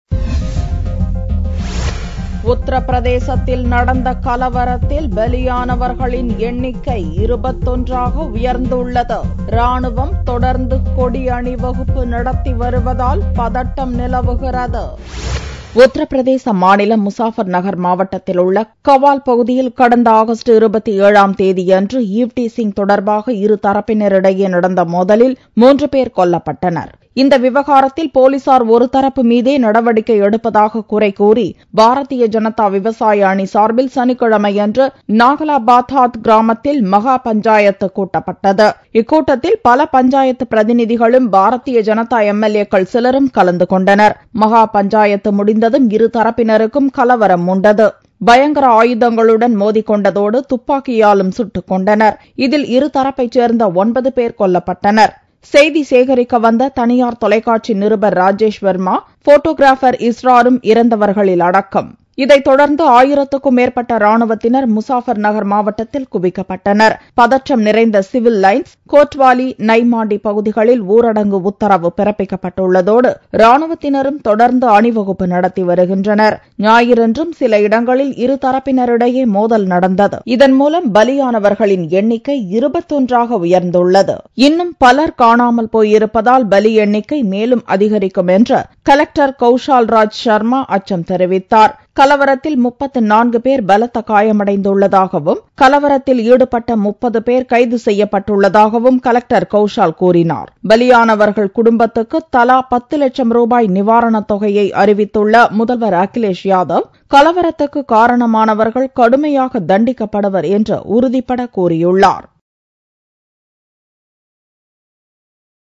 14 Killed in Uttar Pratesh - Dinamalar Sep 8th 2013 News in Tamil Video
14 Killed in Uttar Pratesh - Dinamalar Sep 8th 2013 News in Tamil Video For more videos